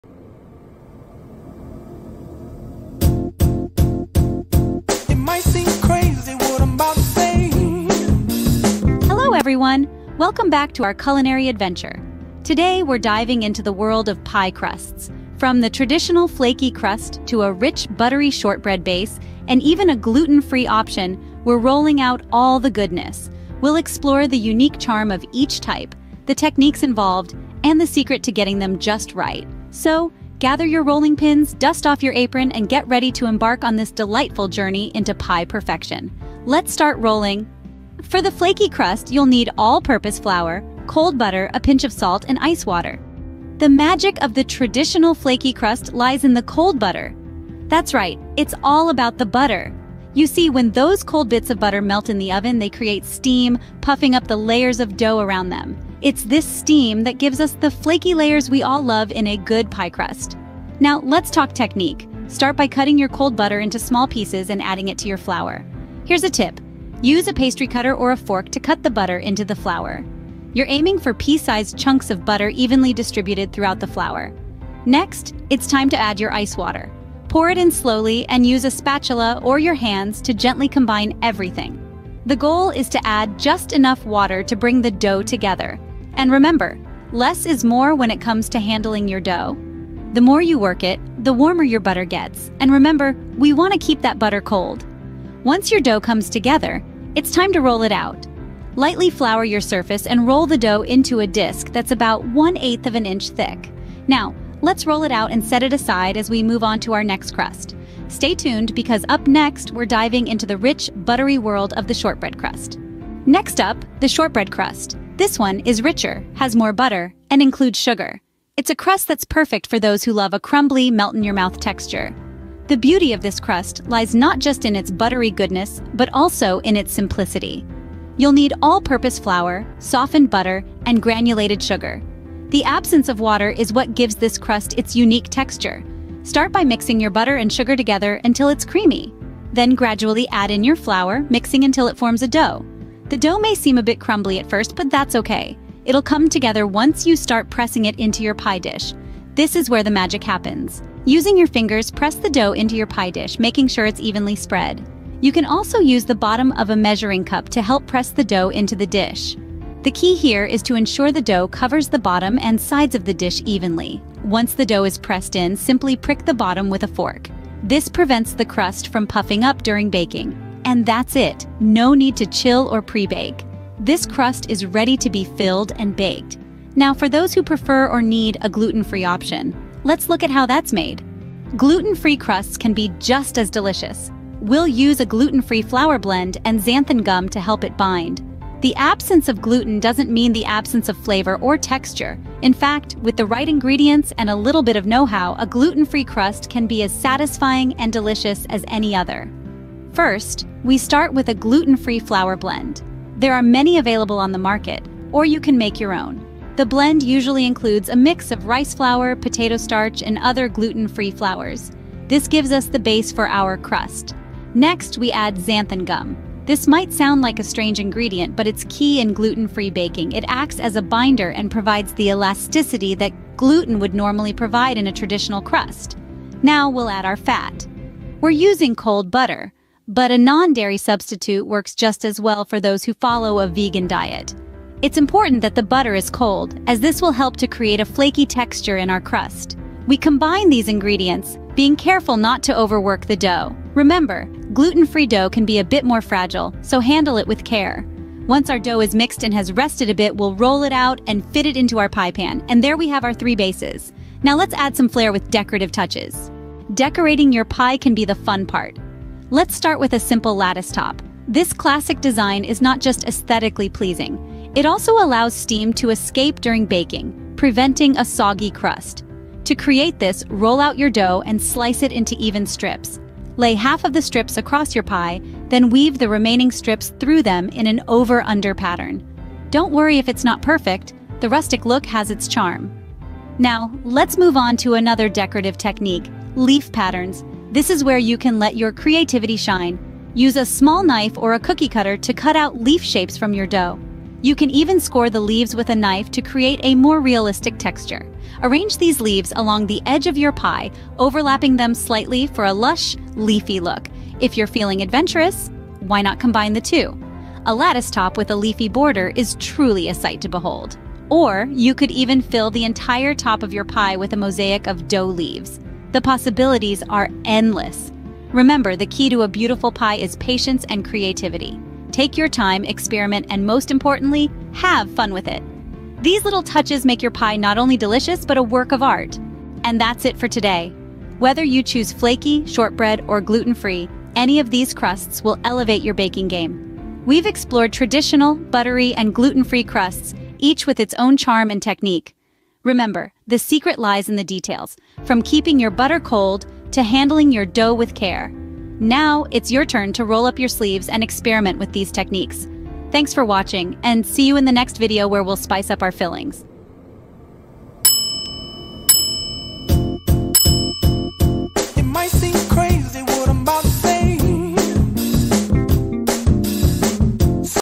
Mastering Pie Crusts: Flaky, Shortbread, & Gluten-Free Options!
Unlock the secrets to the perfect pie crust in today's episode! 🥧💫 Whether you prefer the classic flaky crust, a rich buttery shortbread, or need a gluten-free option, we've got you covered. Join us as we dive into each method, share top tips, and show you how to add creative touches to your pies. Perfect your pastry skills and impress at your next gathering! 🍴✨ Watch, learn, and bake along.

OUTLINE:

00:00:00 Introduction
00:00:28 Traditional Flaky Crust
00:01:44 Buttery Shortbread Crust
00:02:52 Gluten-Free Crust
00:04:07 Decorating Pie Crusts
00:05:20 Conclusion